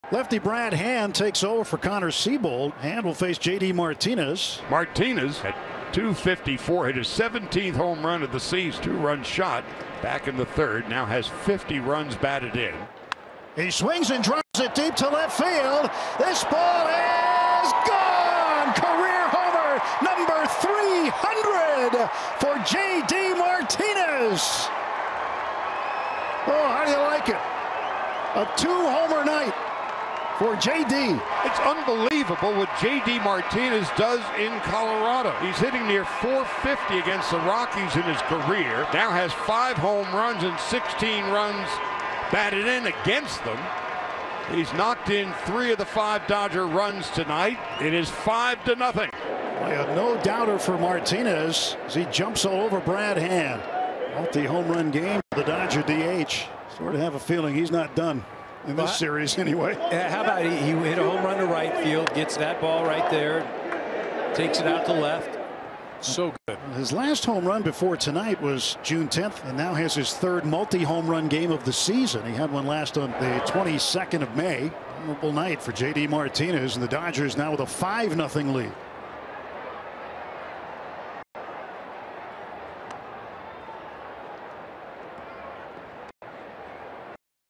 JUST DINGERS x300! J.D. Martinez’s 300th career home run!
J.D. Martinez’s second home run of the game is his 300th career home run.